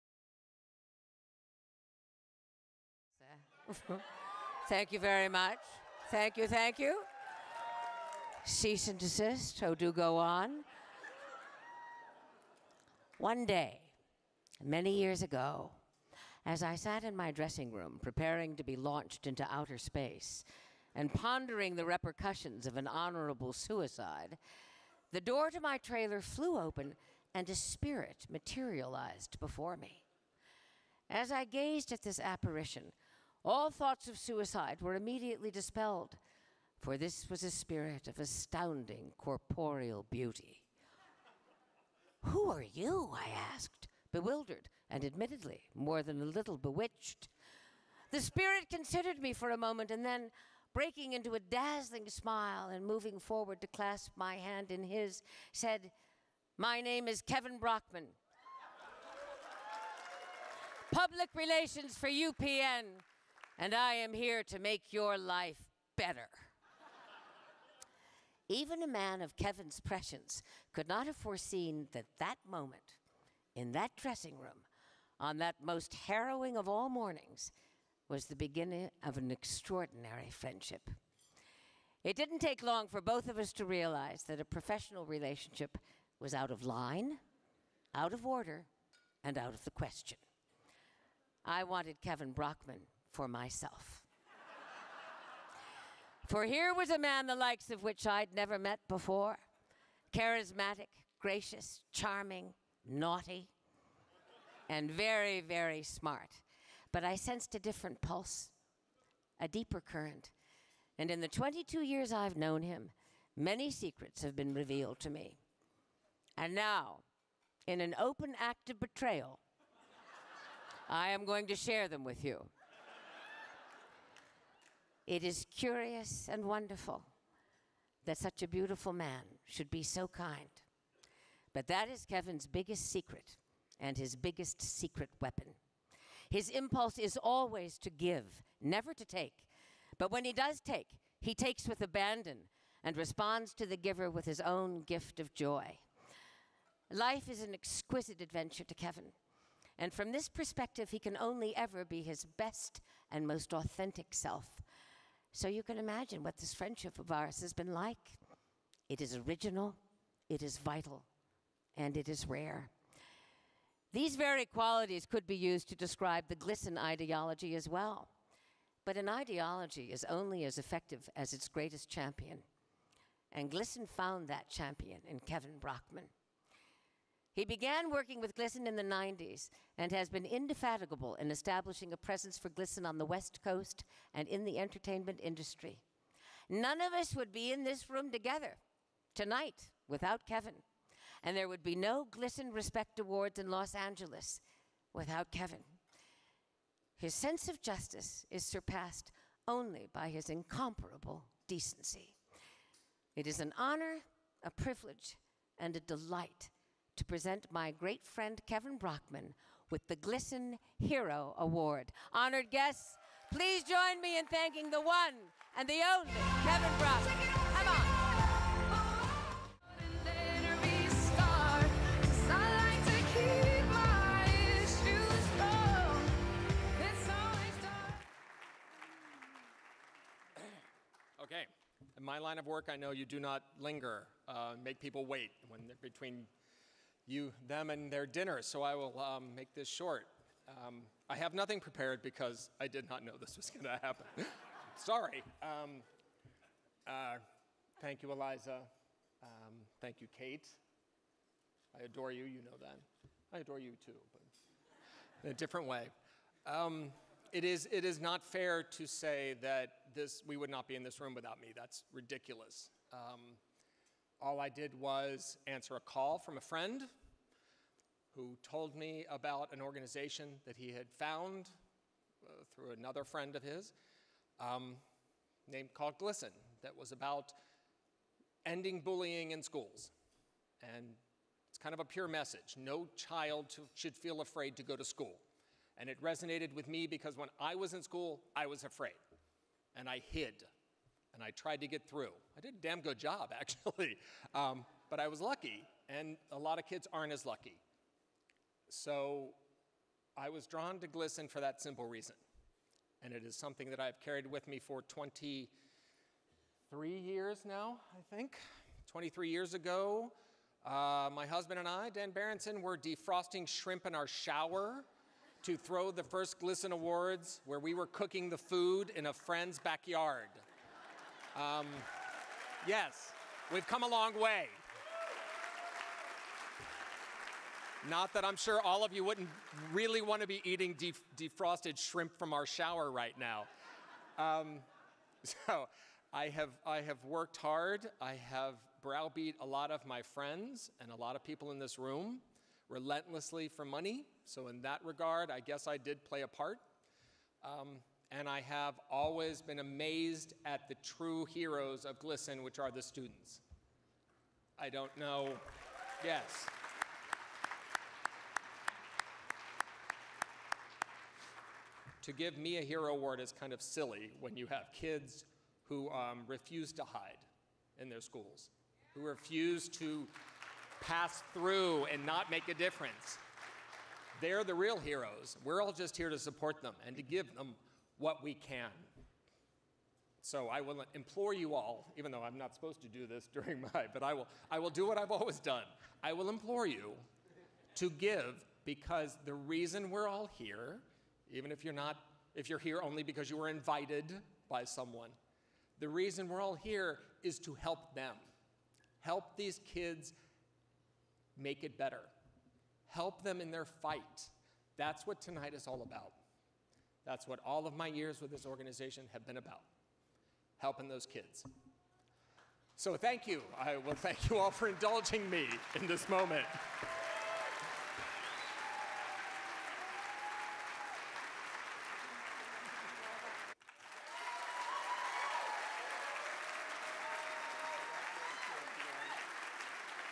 Kate Mulgrew presents Hero Award, GLSEN Respect Awards Los Angeles 2016
Actress Kate Mulgrew presents the Hero Award to Kevin Brockman at the GLSEN Respect Awards - Los Angeles.